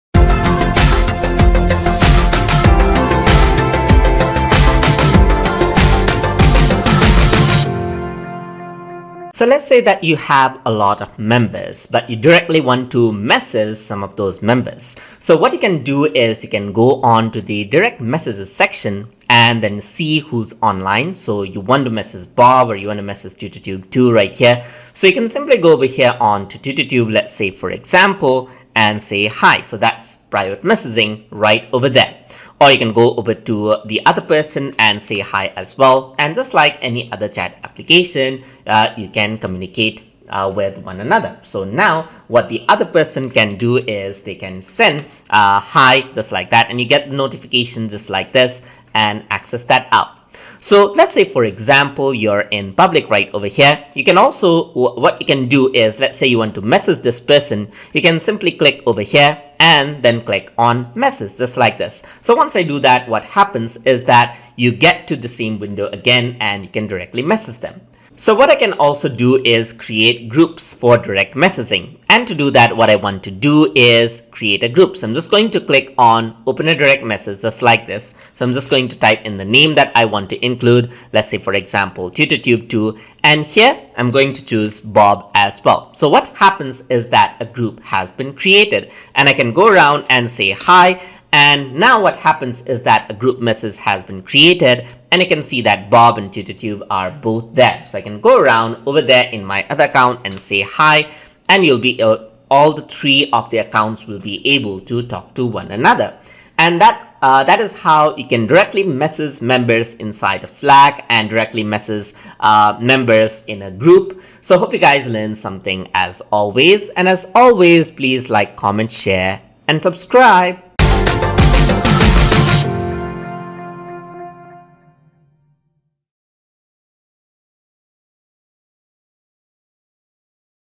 Slack Tutorial - Lesson 14 - Direct Messaging and Creating Message Groups
In this tutorial, we will be discussing about Direct Messeging and Creating Message Groups in Slack

Learn the basics of using Slack including how to communicate, collaborate and share documents with your team members.

This lesson has been made using Slack for Windows 10 but is applicable for Mac Version of the application and web version of the application as well.